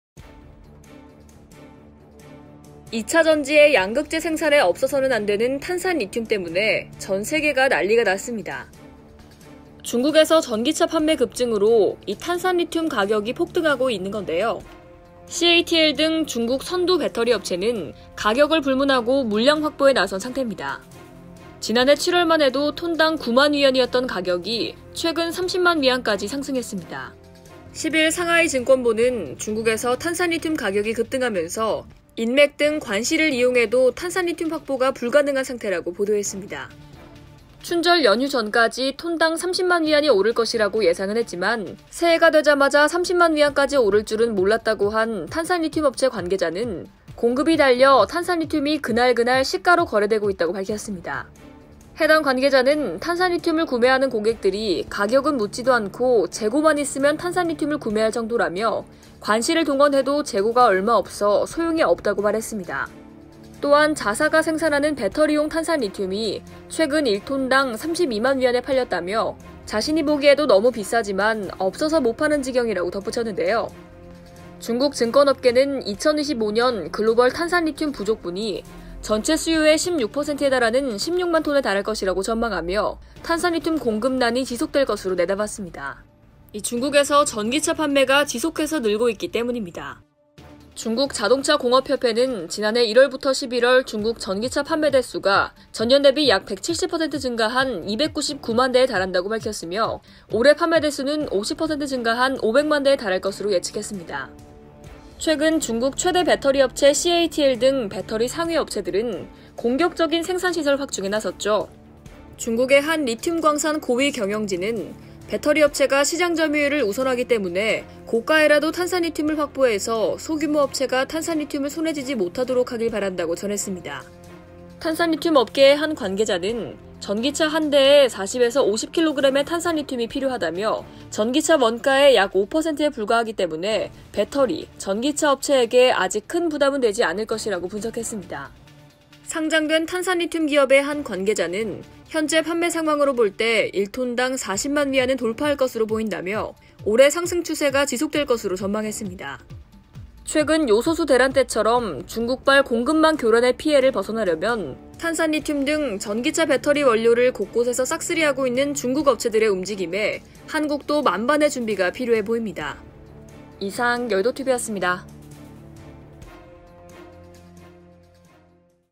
[여의도튜브] 순식간 1620만→5400만원 中 또 ‘공포의 싹쓸이’ /머니투데이방송
2차전지의 양극재 생산에 없어서는 안 되는 탄산리튬 때문에 전 세계가 난리가 났습니다.
중국에서 전기차 판매 급증으로 이 탄산리튬 가격이 폭등하고 있는 건데요. CATL 등 중국 선두 배터리업체는 가격을 불문하고 물량 확보에 나선 상태입니다.

아나운서: 선소연
영상콘텐츠: 길인호
기사콘텐츠: 선소연
기획: 강인형
총괄: 정미경
제작: MTN 머니투데이방송

- MTN 투데이